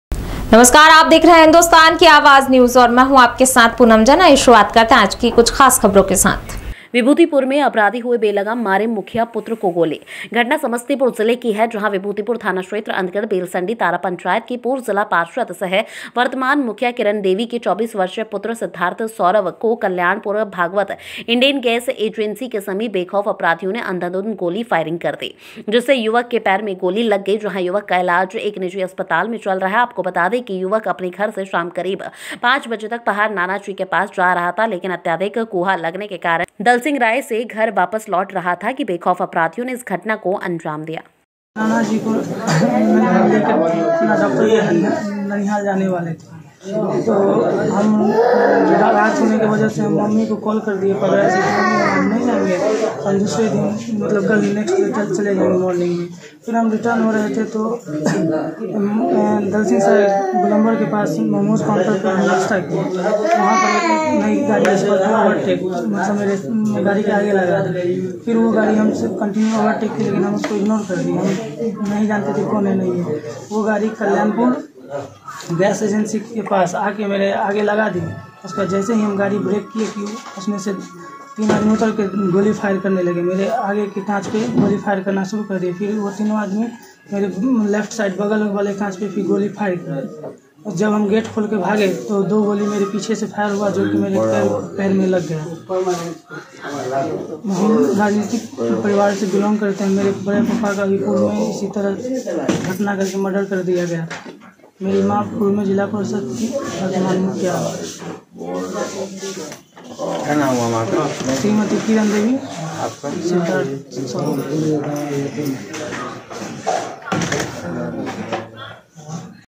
बिहार बिभूतिपुर में अपराधी द्वारा मुखिया पुत्र को गोली मारने की घटना को लेकर सड़क को जाम
मारे मुखिया पुत्र को गोली। आक्रोशित ग्रामीणों ने किया सड़क जाम।
समस्तीपुर संवाददाता संतोष राज की रिपोर्ट

बिभूतिपुर में अपराधी द्वारा  मुखिया पुत्र को गोली मारने की  घटना को लेकर सड़क को जाम कर दिए।
 समस्तीपुर जिले की है जहां बिभूतिपुर थाना क्षेत्र अंतर्गत बेलसंडी तारा पंचायत की पूर्व जिला पार्षद सह वर्तमान मुखिया किरण देवी के 24 वर्षीय पुत्र सिद्धार्थ सौरव को कल्याणपुर भागवत इंडेन गैस एजेंसी के समीप बेखौफ अपराधियों ने अंधाधुंध गोली फायरिंग करना शुरू कर दी। जिससे युवक के पैर गोली लग गई। जहां युवक का ईलाज एक निजी अस्पताल में चल रही है। जिससे आक्रोशित ग्रामीणों ने समस्तीपुर रोसड़ा के मुख्य पथ को घंटो भर जाम कर आवागबन बाधित  किया।
 वहीं अपराधियों  की जल्द ही गिरफ्तारी करने की मांग कर रहे थे।अंत में थानाध्यक्ष कृष्ण चंद्र भारती के आश्वासन पर जाम हटाय गया।
आपको बता दें कि युवक अपने घर से शाम करीब 5:00 बजे चक पहार नाना जी के पास जा रहा था लेकिन अत्यधिक कुहा लगने के कारण वह दलसिंहसराय से घर वापस लौट रहा था की बेखौफ अपराधियों ने इस घटना को अंजाम दिया।